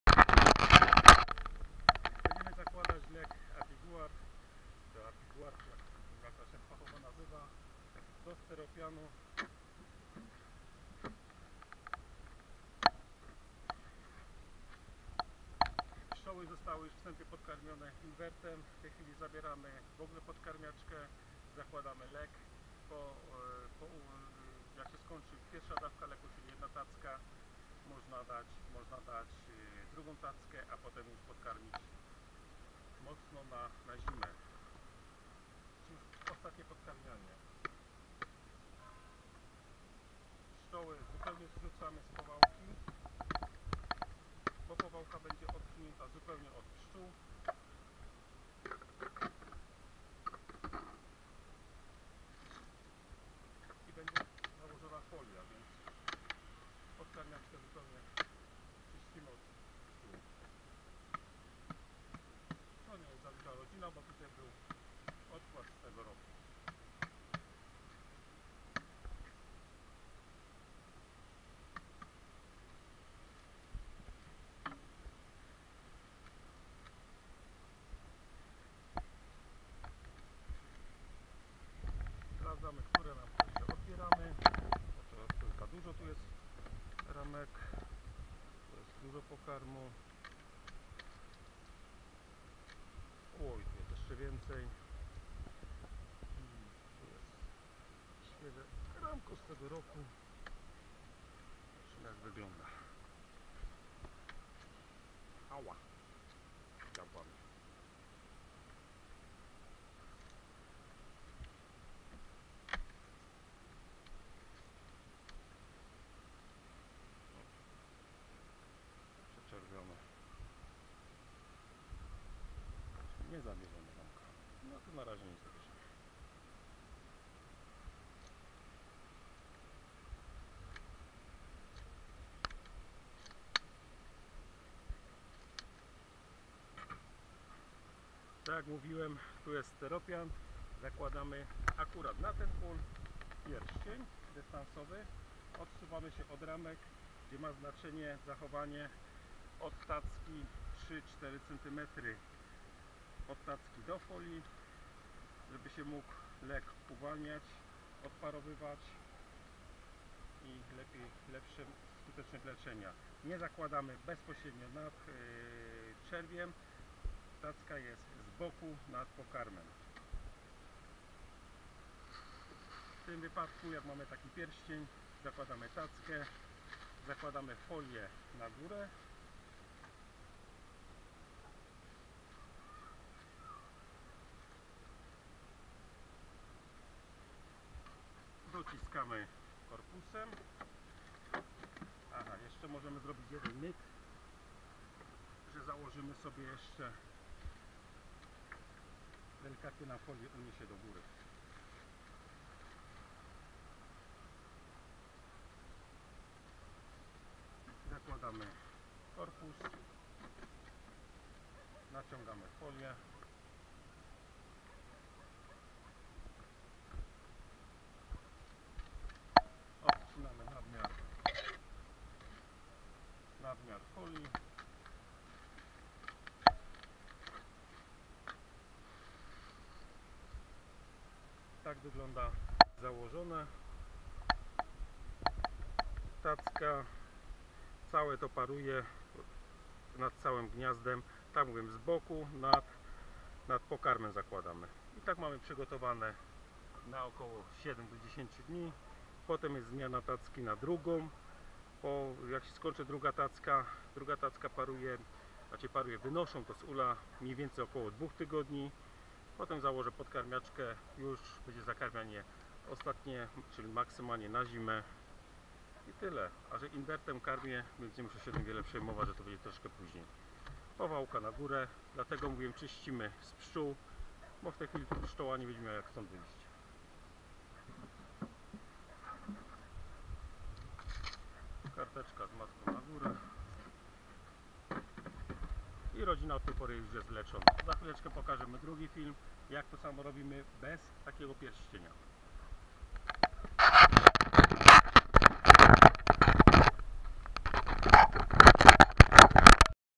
Jak zostosować APIGUARD z pierścieniem
Jak poddać APIGUARD do rodziny pszczelej z zastosowaniem pierścienia dystansowego.